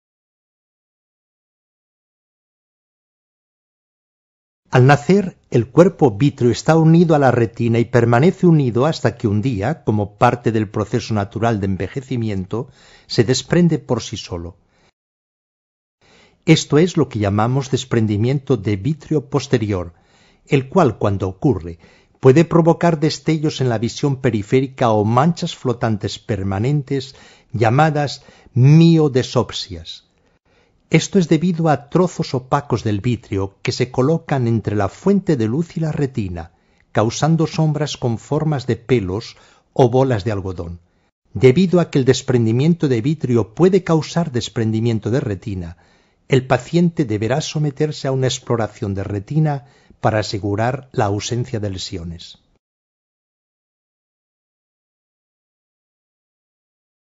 Desprendimiento de vítreo posterior - evrs
Al nacer el cuerpo vítreo está unido a la retina y permanece unido hasta que un día, como parte del proceso natural de envejecimiento, se desprende por si solo.